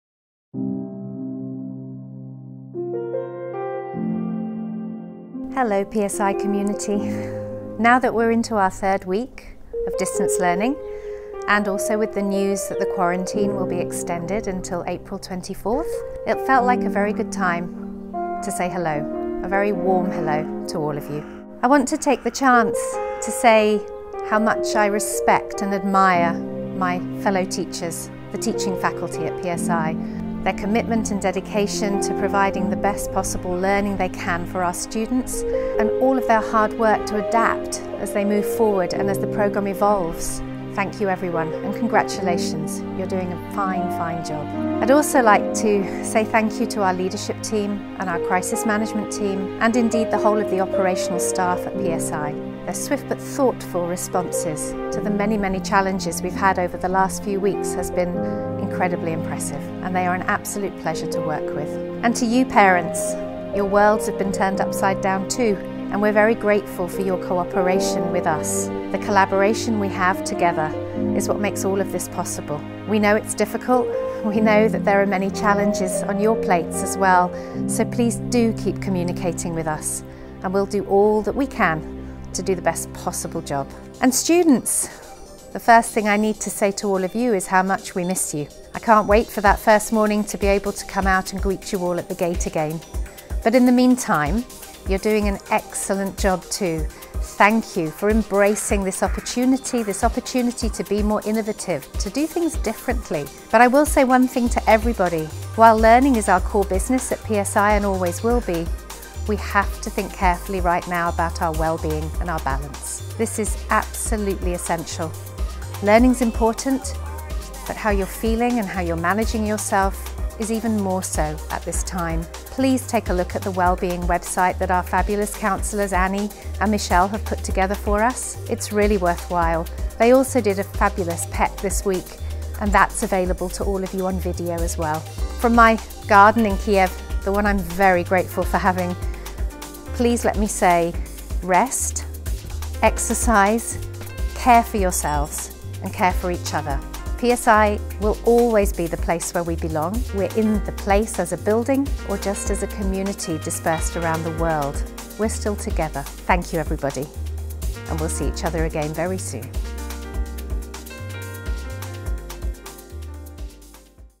PSI Directors's Message / Quarantine 2020
Dear PSI Community, please enjoy watching our latest video update from the PSI Director, Rachel Caldwell.